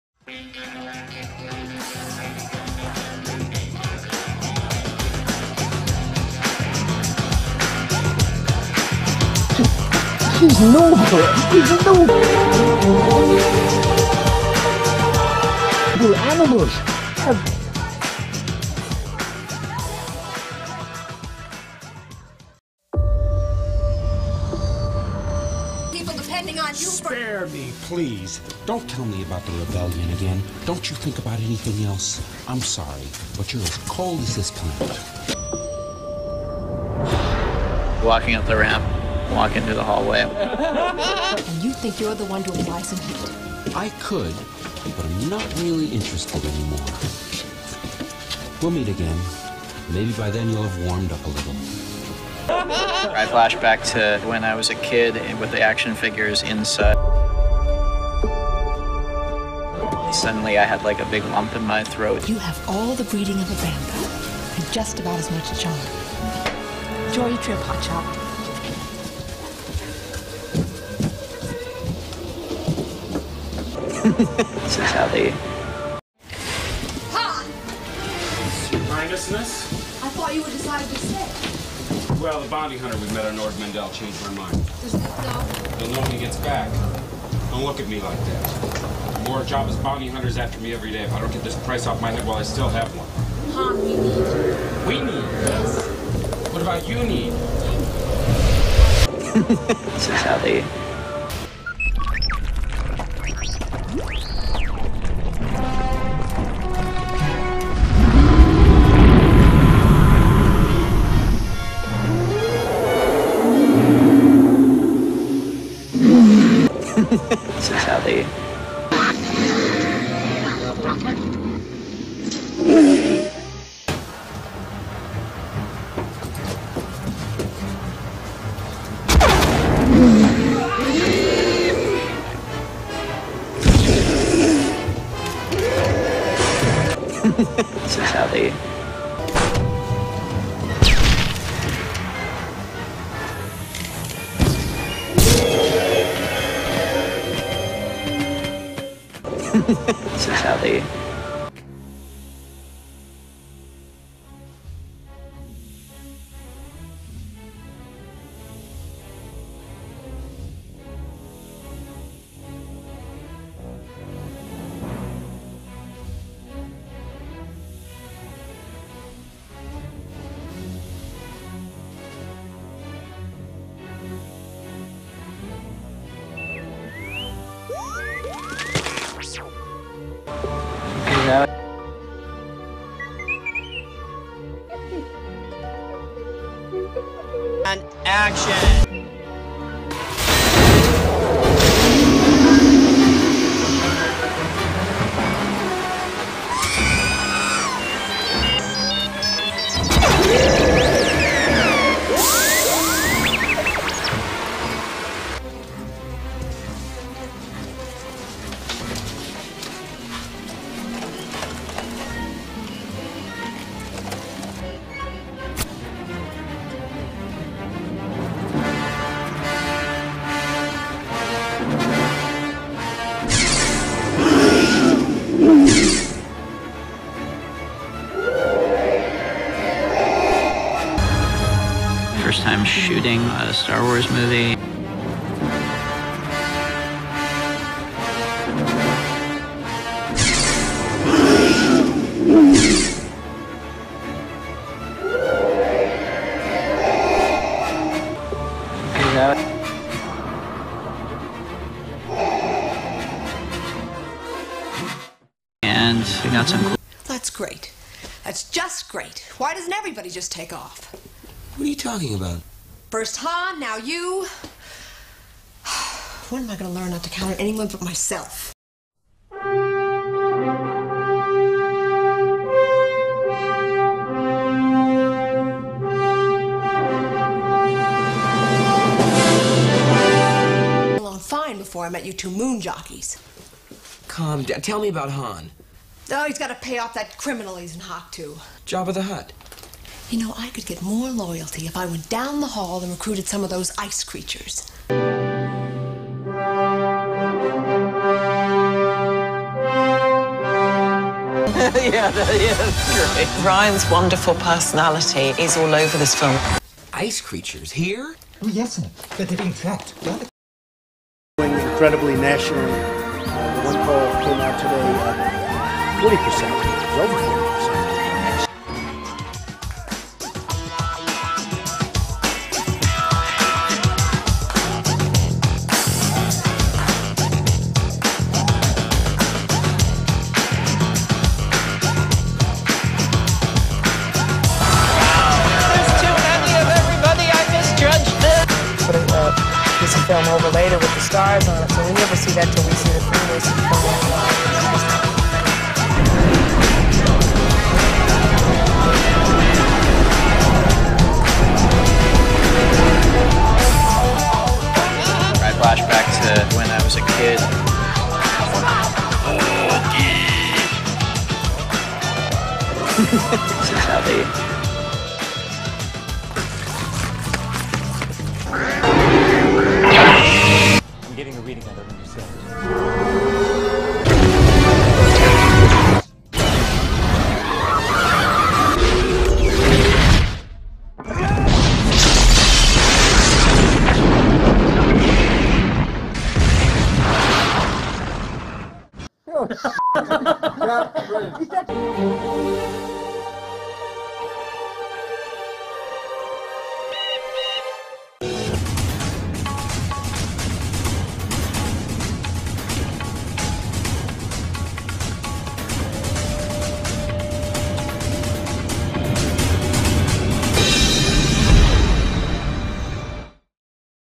Rian 'WAMPA' Johnson by Real Neo (HD)1080p
Hi h guys! Here our OT heroes must deal with the new 'EU' monsters. Enjoy and 1Love to all involved here!!!